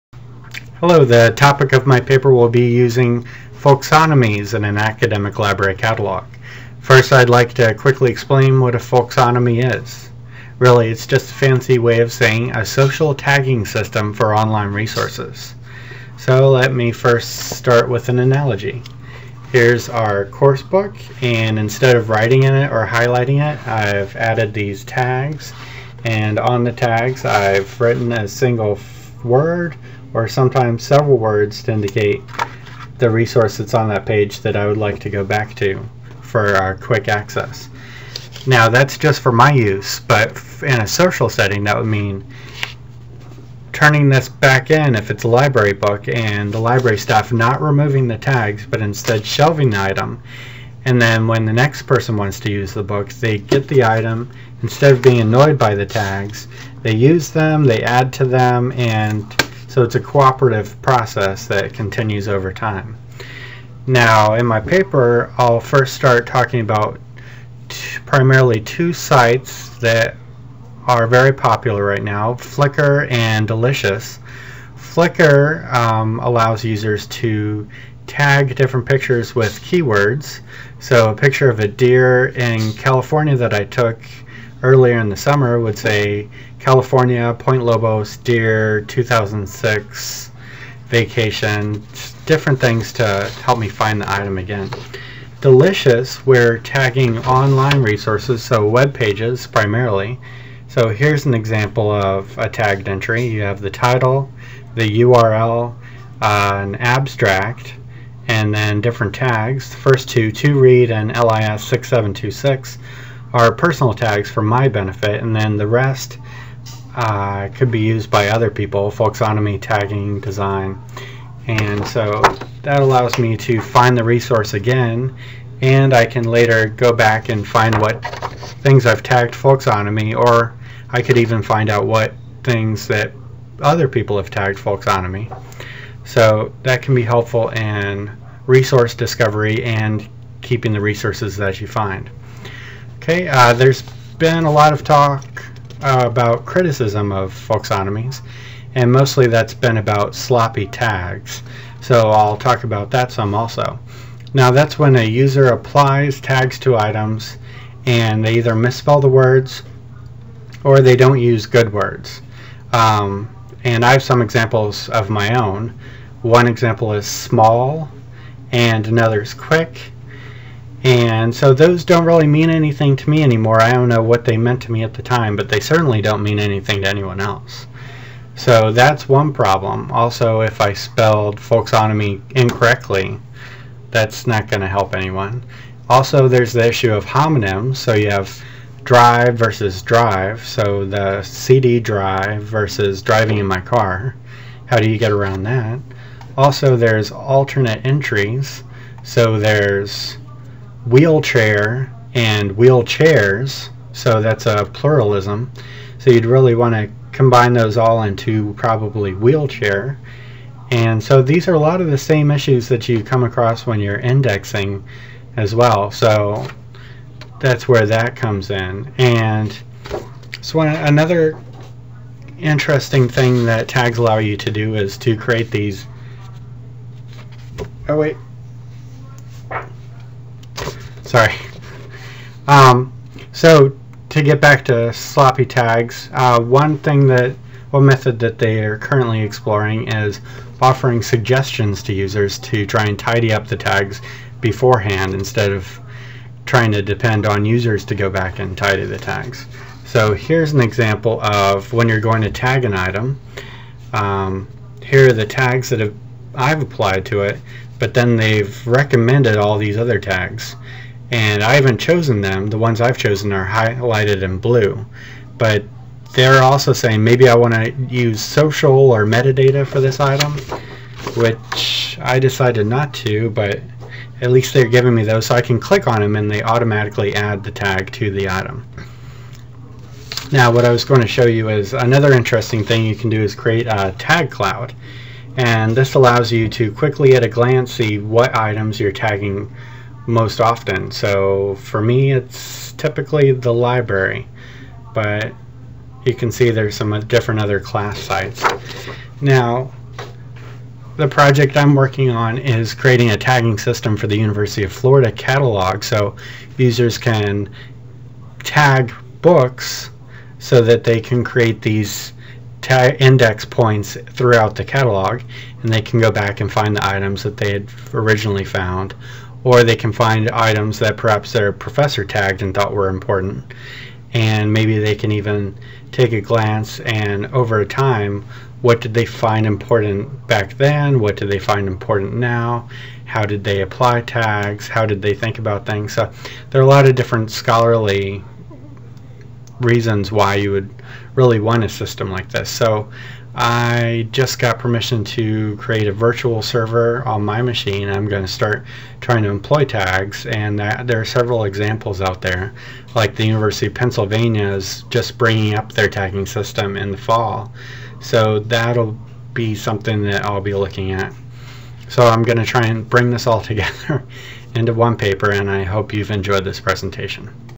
Folksonomy Paper
This is my presentation to describe my final project in LIS 6726. The topic is "Using the use of Folksonomies in an Academic Library Setting".